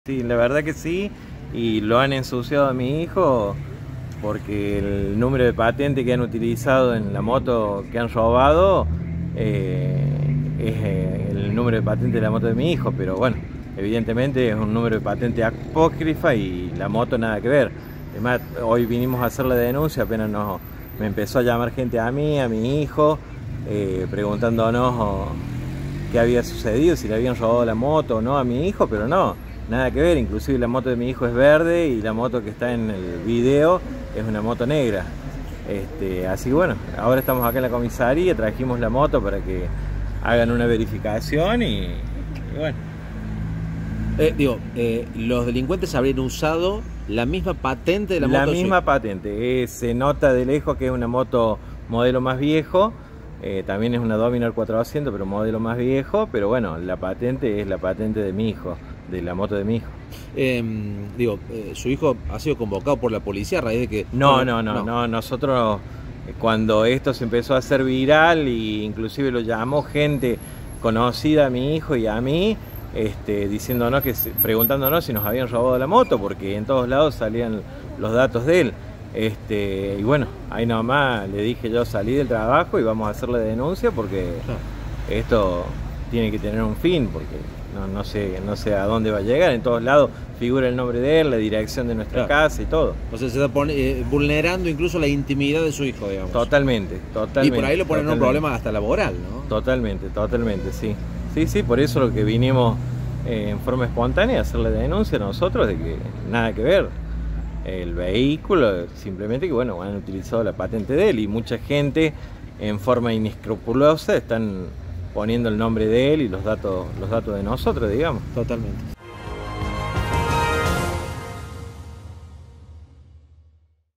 𝗟𝗢 𝗔𝗖𝗨𝗦𝗔𝗥𝗢𝗡 𝗙𝗔𝗟𝗦𝗔𝗠𝗘𝗡𝗧𝗘 𝗗𝗘 𝗨𝗡 𝗥𝗢𝗕𝗢 𝗤𝗨𝗘 𝗡𝗢 𝗖𝗢𝗠𝗘𝗧𝗜Ó
Los delincuentes utilizaron su número de patente
clonado para perpetrar un asalto con la modalidad
motochorro.
JORGE CAMINO - Padre de la víctima